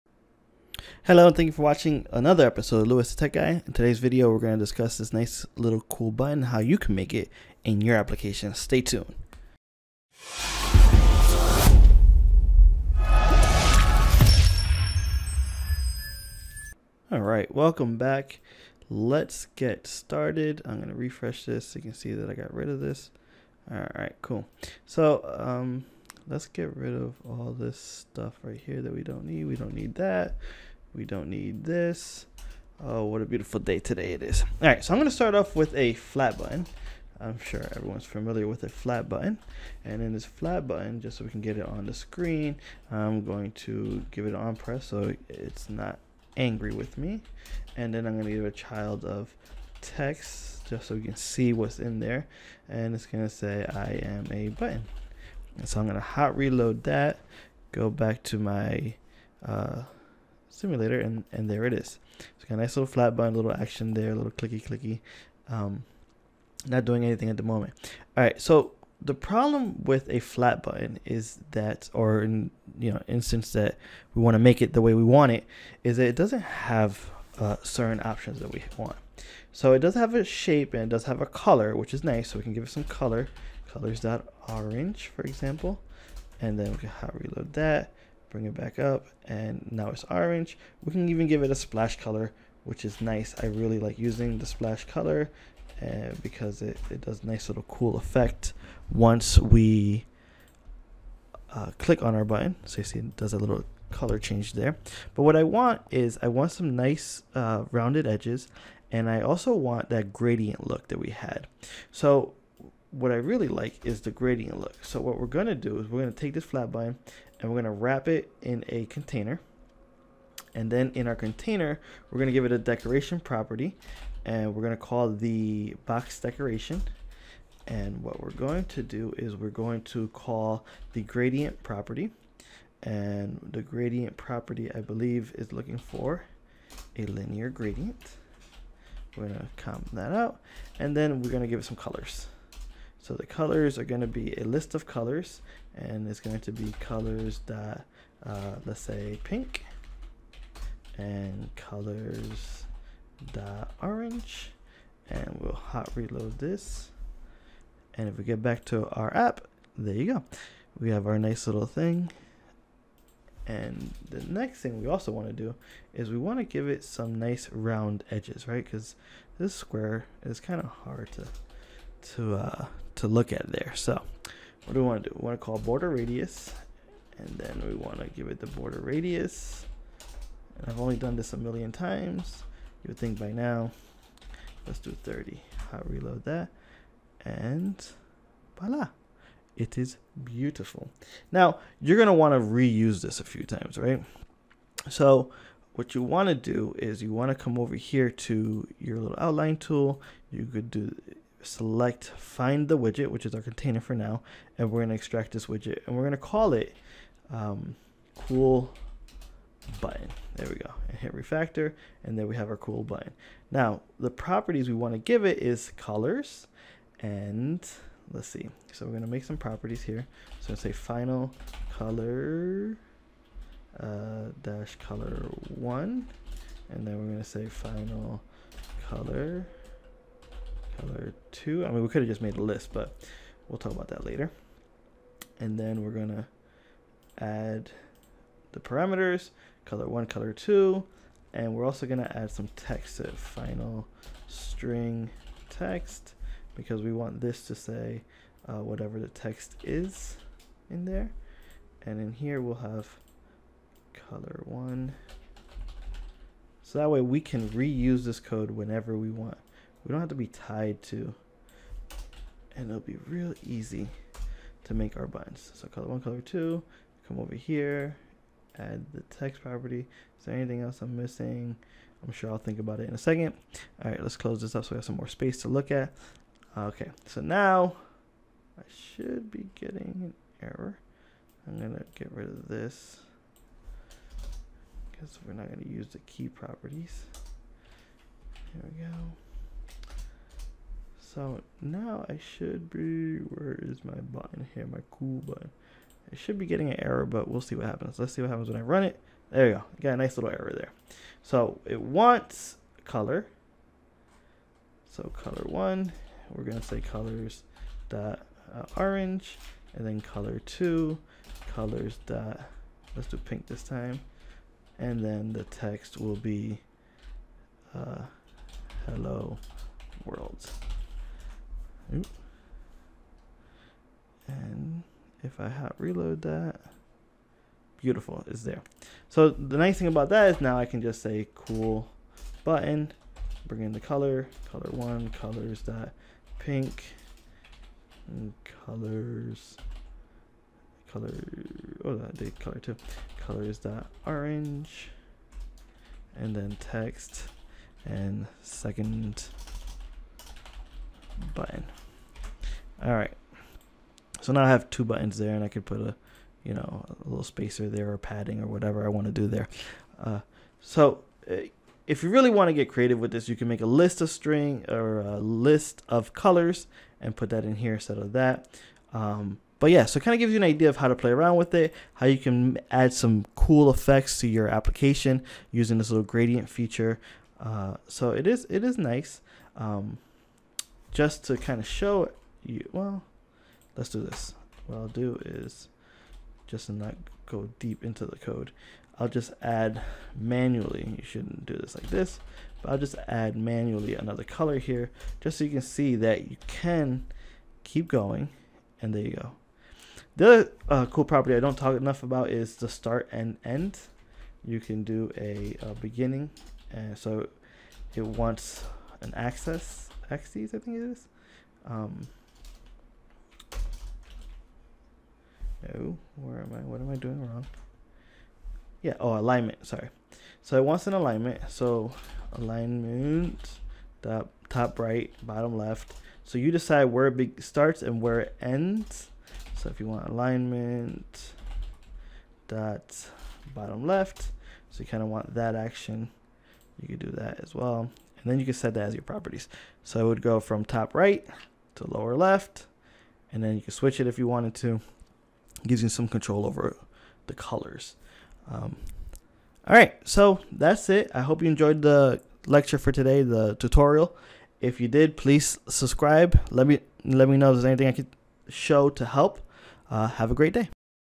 Flutter! Custom Button Widget
In today's video, we will review how to create a custom button in Flutter and how to add a gradient background to it.